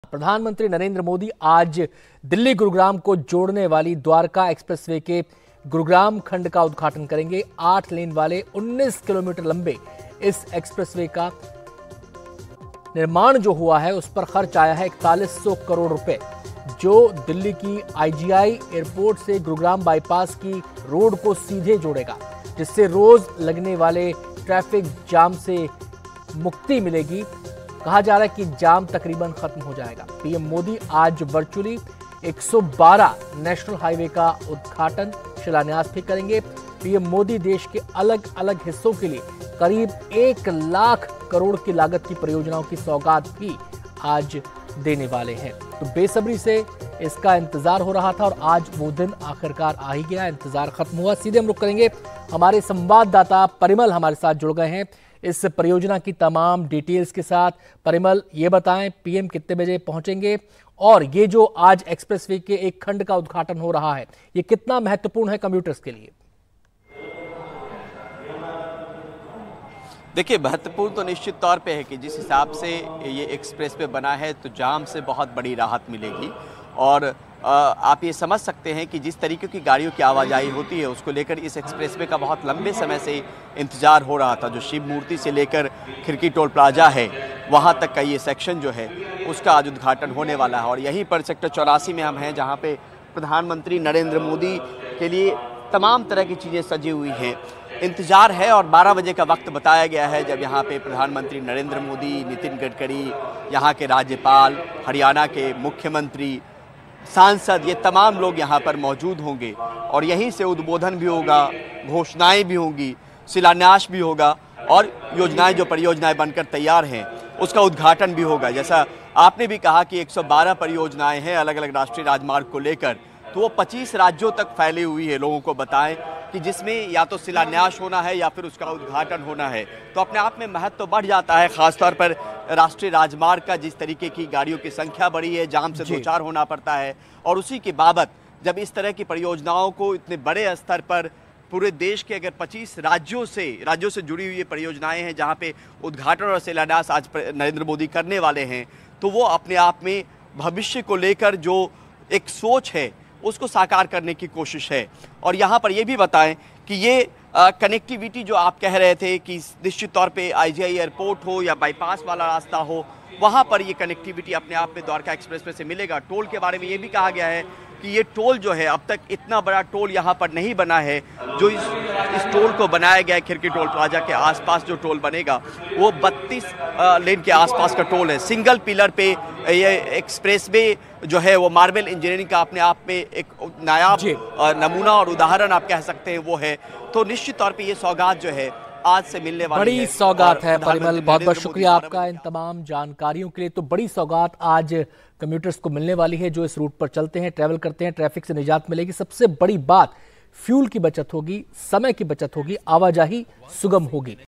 Dwarka Expressway Inauguration: PM Modi का Gurugram दौरा, Dwarka Expressway का करेंगे उद्घाटन
: प्रधानमंत्री नरेंद्र मोदी आज हरियाणा के गुरुग्राम के दौरे पर रहेंगे. वह दोपहर को 12 बजे यहां पर 112 नेशनल हाईवे प्रोजेक्ट्स का शिलान्यास और उद्घाटन करेंगे, जो पूरे देशभर में फैले होंगे और इनकी कीमत लगभग एक लाख करोड़ रुपये है.